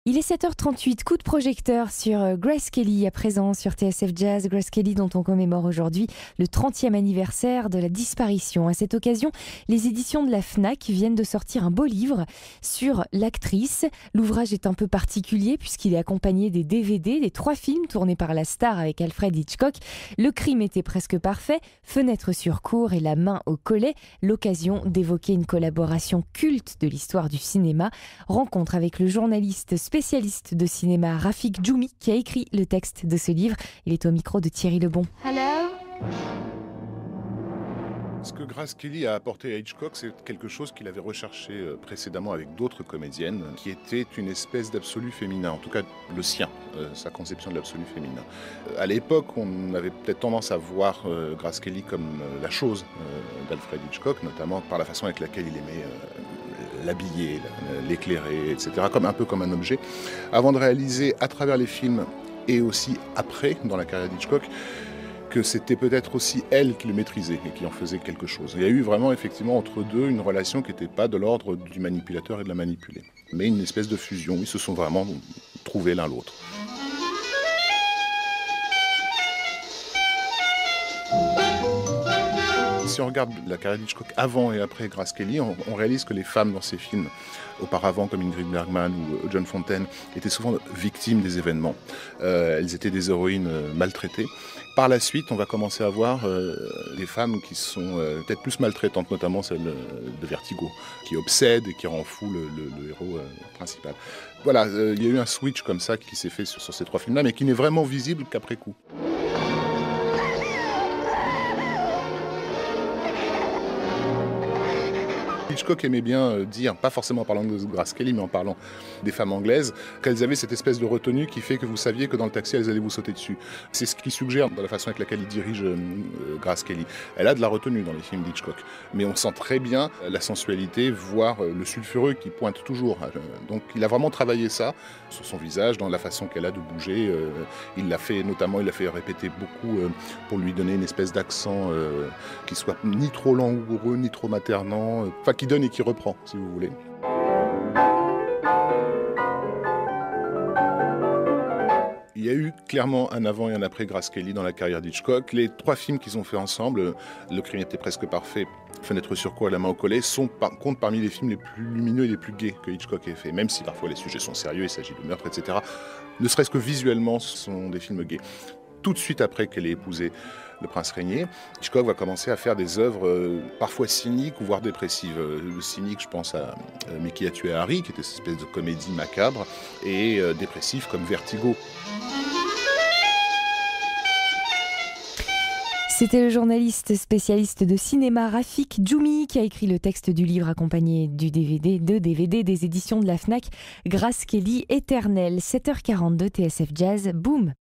Rafik Djoumi parle de Grace Kelly
Intervention sur TSF Jazz le 14/09/2012